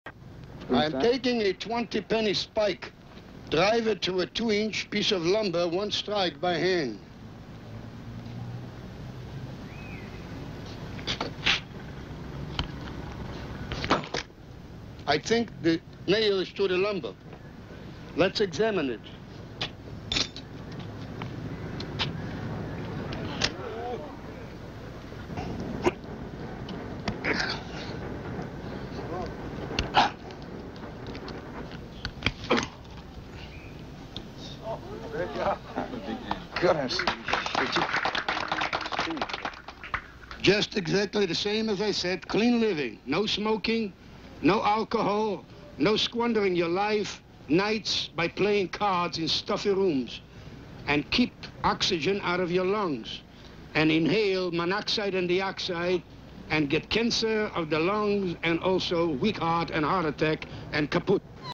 and caput (vintage side show)
Vintage street side show of man driving nail with hand